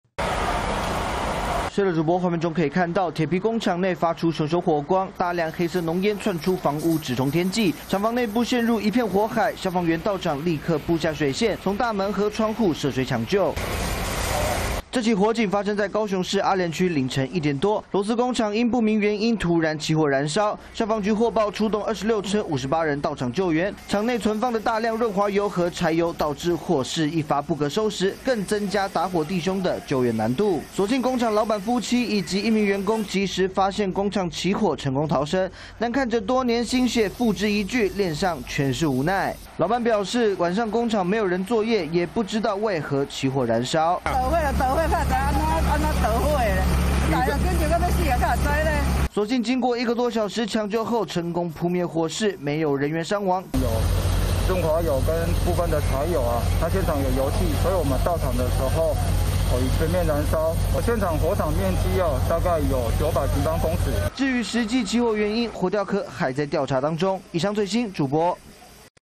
螺絲工廠凌晨大火 老闆3人及時逃出無傷亡│中視新聞 20220602
高雄市阿蓮區一間螺絲工廠凌晨一點多因不明原因起火，由於廠內存放大量潤滑油及柴油，導致火勢迅速蔓延。所幸工廠老闆夫妻和一位員工及時逃生，沒有人員傷亡。詳細情況連線記者 張至人。